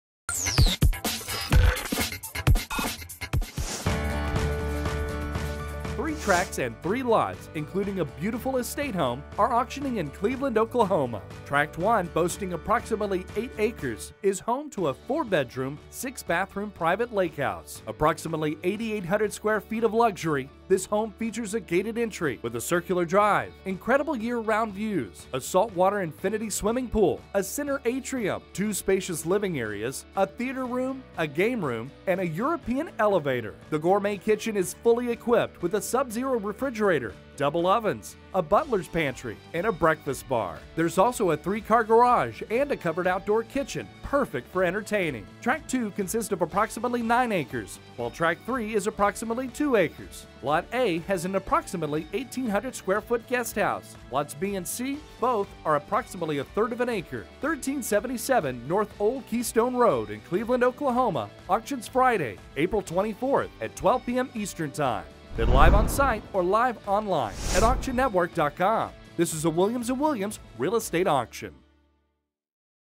Home Auction in Cleveland, Oklahoma
Home Auction in Cleveland, OK
1377 North Old Keystone Road auctions
April 24, 2015 at 11:00a CT

Beautiful Estate Home with a European flair. This very private lakehouse features a gated entry with a circular drive and is move-in ready and has 8800+/- sf of luxury with incredible year round views, saltwater infinity swimming pool, center atrium, 4 huge bedrooms 6 bathrooms, 3 car garage, 2 spacious living areas, theater room, game room and european elevator. The gourmet kitchen is fully equipped with a sub zero refrigerator, double ovens, butler pantry and a breakfast bar. There's also a covered outdoor kitchen perfect for entertaining! PropertyID: 340677